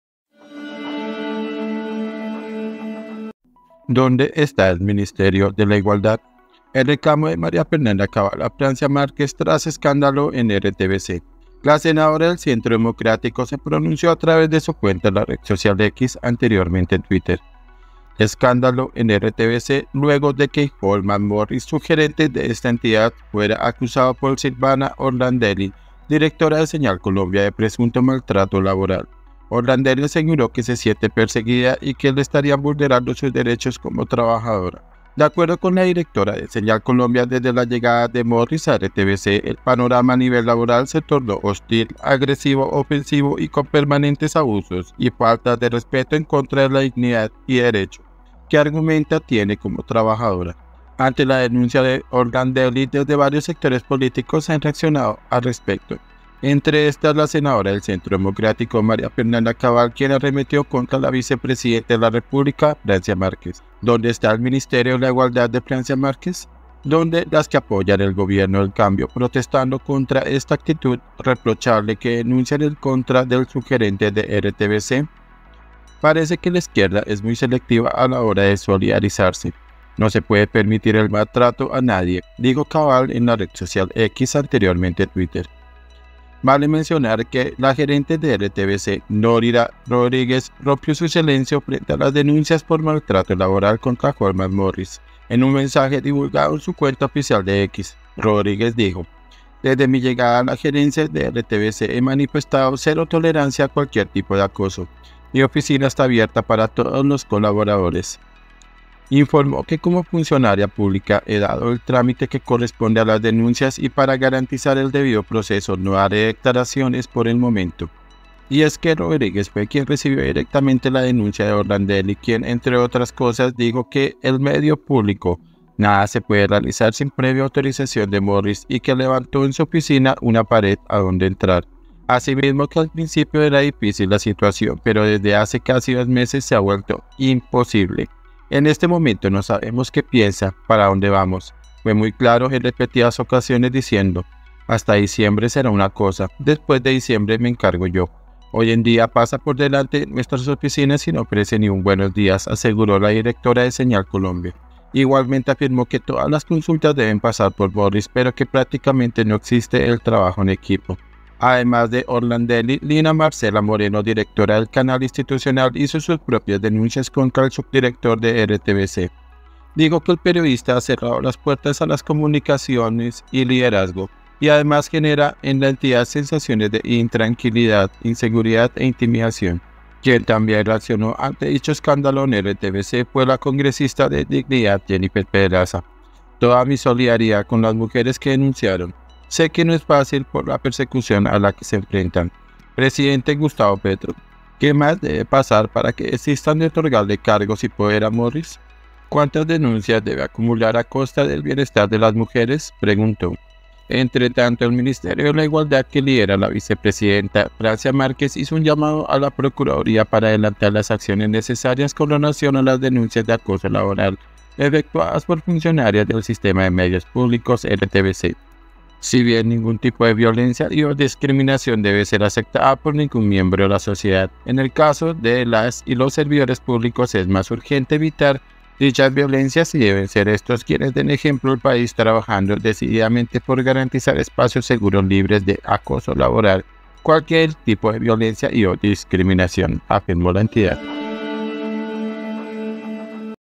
📢 EL RECLAMO DE MARIA FERNANDA CABAL A FRANCIA MÁRQUEZ TRAS ESCÁNDALO EN RTVC
“¿Dónde está el Ministerio de la Igualdad de Francia Márquez? ¿dónde las que apoyan el gobierno del “Cambio” protestando contra esta actitud reprochable que denuncian en contra el subgerente de RTVC? Parece que la izquierda es muy selectiva a la hora de solidarizarse. No se puede permitir el maltrato a nadie”, dijo Cabal en la red social X, anteriormente Twitter.
SEMANA